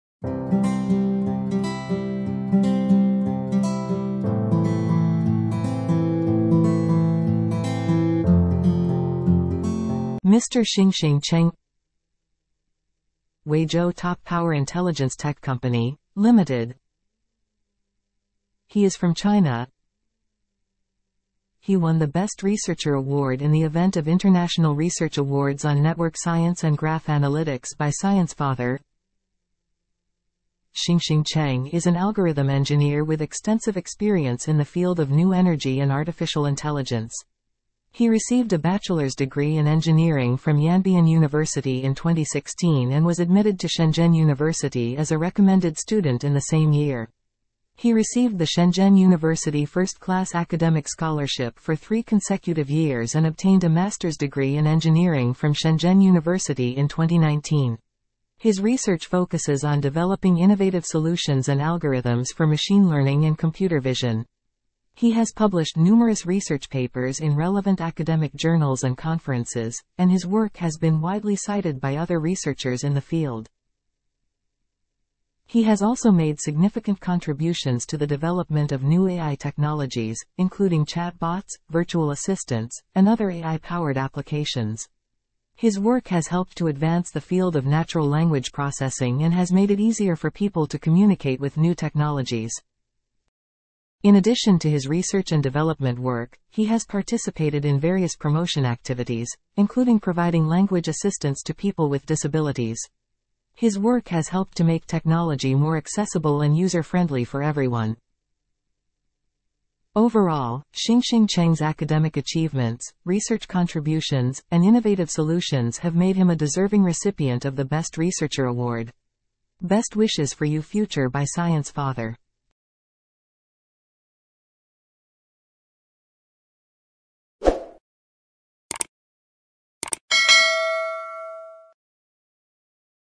Mr. Xingxing Cheng | Huizhou Top Power Intelligence Tech Co., Ltd.| China | Best researcher award
Xingxing Cheng is an algorithm engineer with extensive experience in the field of new energy and artificial intelligence. He received a Bachelor's degree in Engineering from Yanbian University in 2016 and was admitted to Shenzhen University as a recommended student in the same year. He received the Shenzhen University First-Class Academic Scholarship for three consecutive years and obtained a Master's degree in Engineering from Shenzhen University in 2019.

His research focuses on developing innovative solutions and algorithms for machine learning and computer vision. He has published numerous research papers in relevant academic journals and conferences, and his work has been widely cited by other researchers in the field.

He has also made significant contributions to the development of new AI technologies, including chatbots, virtual assistants, and other AI-powered applications. His work has helped to advance the field of natural language processing and has made it easier for people to communicate with new technologies.

In addition to his research and development work, he has participated in various promotion activities, including providing language assistance to people with disabilities. His work has helped to make technology more accessible and user-friendly for everyone.

Overall, Xingxing Cheng's academic achievements, research contributions, and innovative solutions have made him a deserving recipient of the Best Researcher Award.